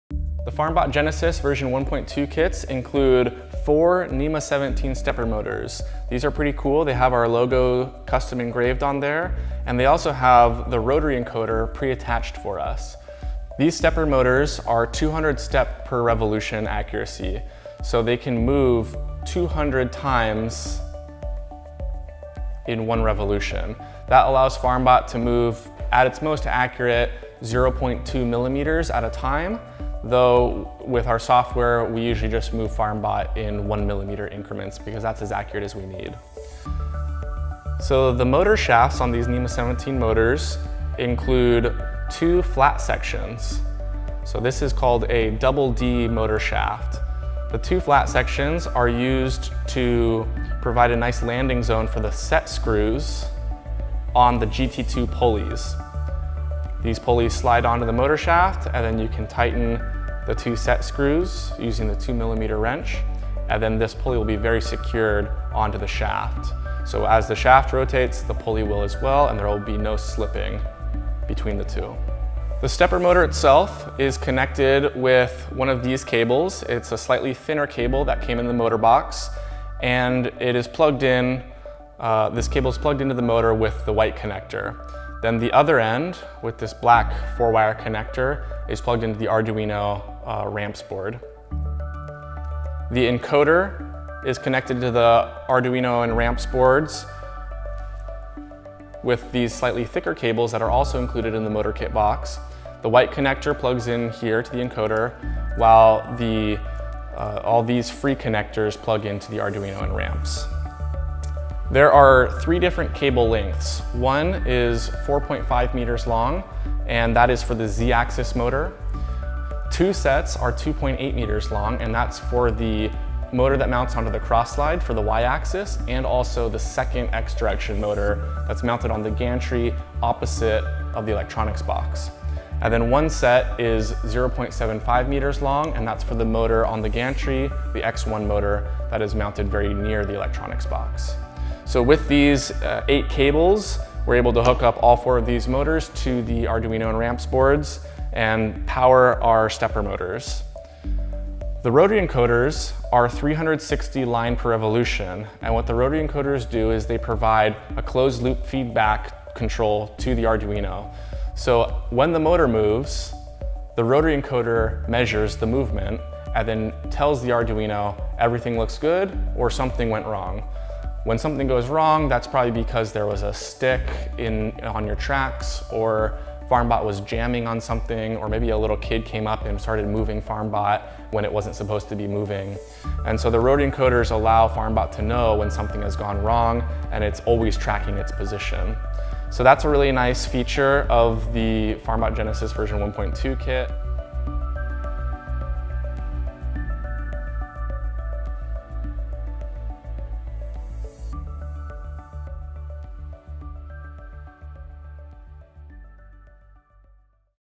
NEMA 17 Stepper Motors and Rotary Encoders - FarmBot Genesis v1.2
Learn about the NEMA 17 Stepper Motors and Rotary Encoders used in FarmBot Genesis v1.2. For complete tech specs, 3D models, and more,